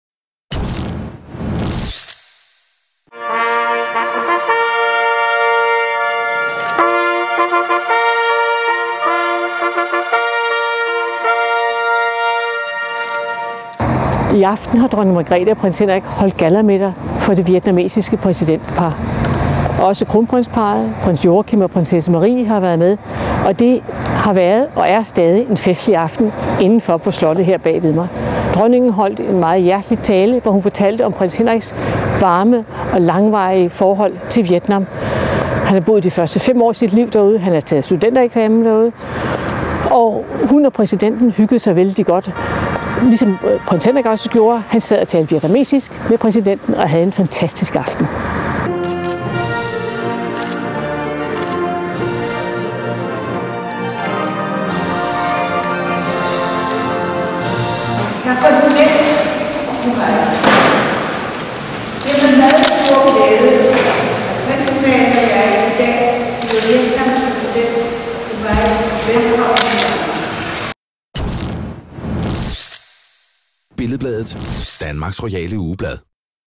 Gallataffel på Fredensborg Slot
BILLED-BLADET var med, da regentparret onsdag aften afholdt gallataffel i anledningen af statsbesøget fra Vietnam.